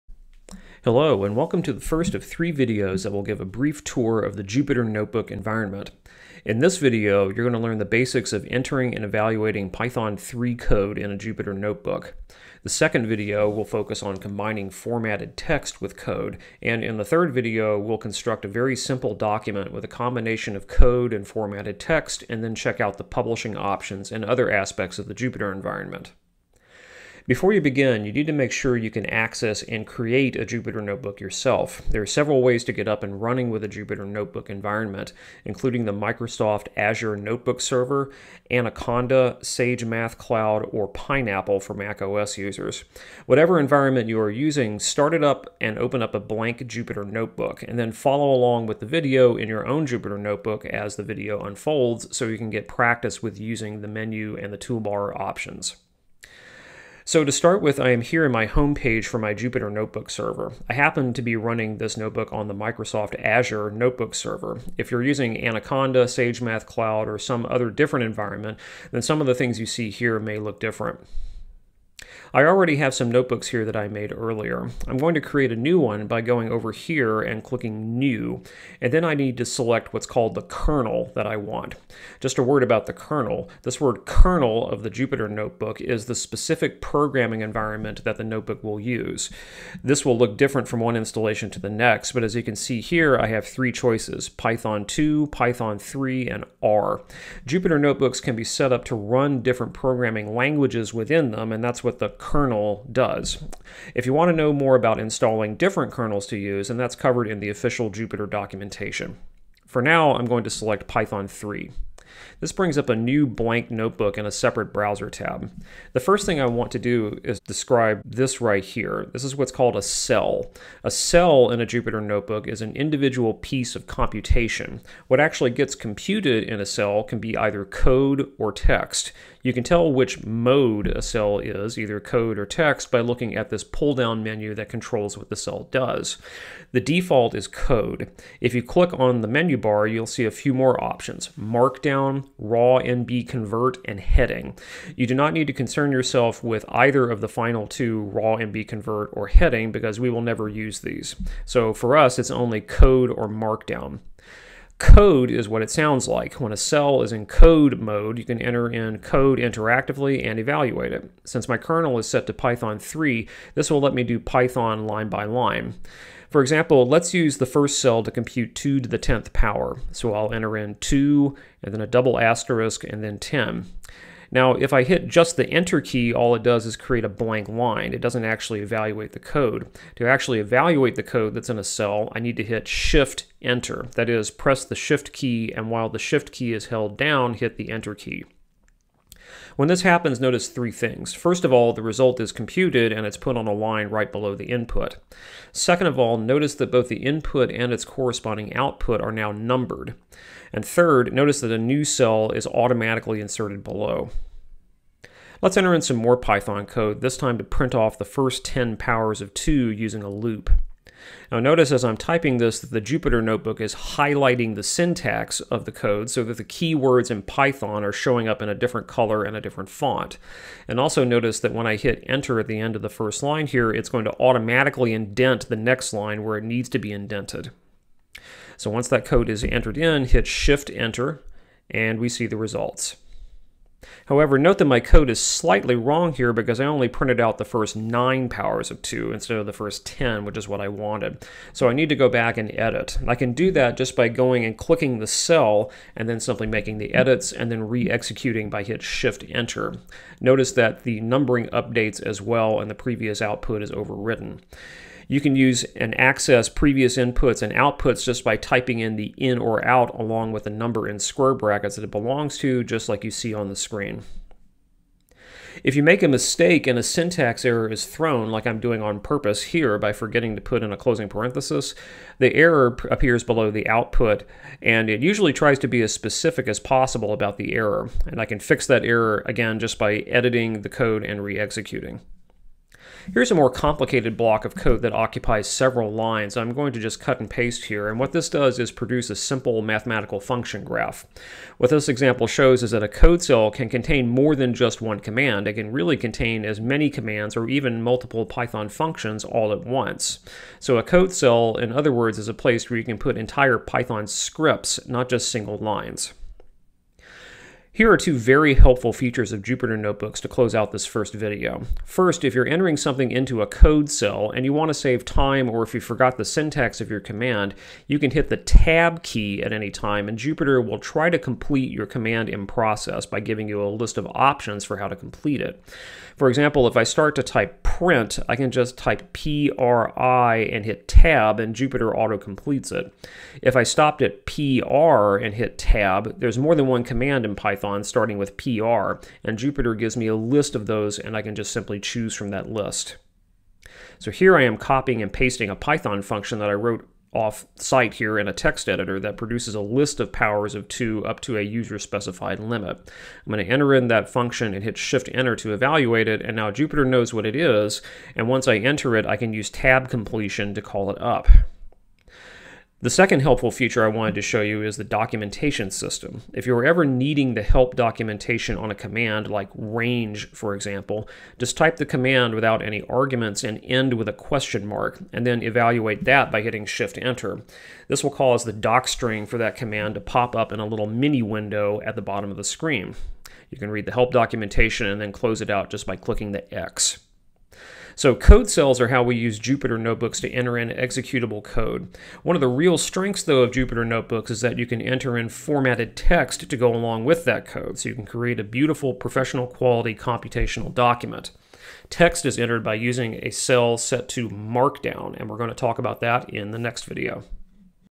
The Jupyter Notebook Environment part 1
Part 1 of  three-part video series on getting started with the Jupyter notebook environment. This one covers working with Python code.